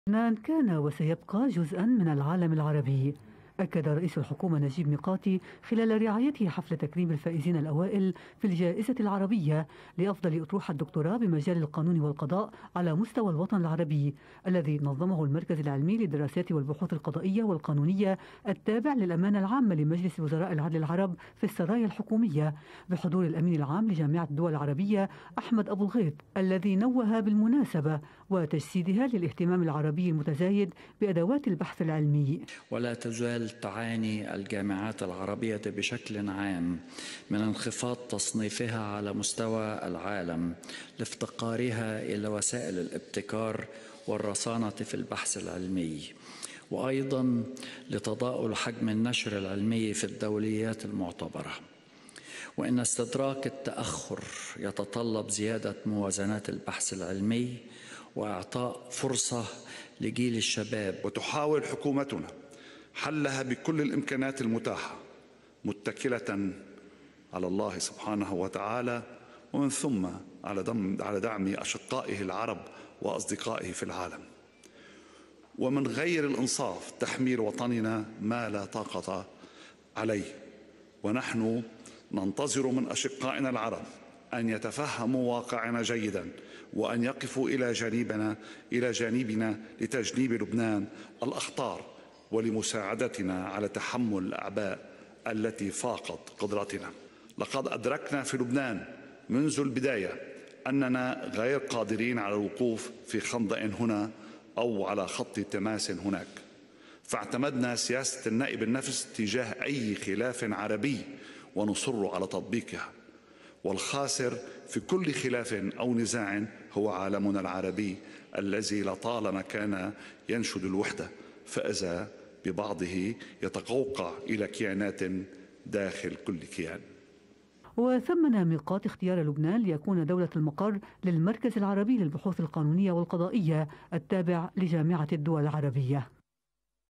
نجيب ميقاتي يؤكد اصرار الحكومة على تطبيق النأي بالنفس ويناشد الاشقاء العرب لدعم لبنان في ازمــته
أكد رئيس الحكومة اللبنانية نجيب ميقاتي إصرار حكومته على تطبيق سياسة النأي بالنفس، ودعا الدول العربية إلى الوقوف إلى جانب لبنان ومساعدته لتحمل الأعباء، مؤكداً أنه كان وسيبقى جزءاً من العالم العربي.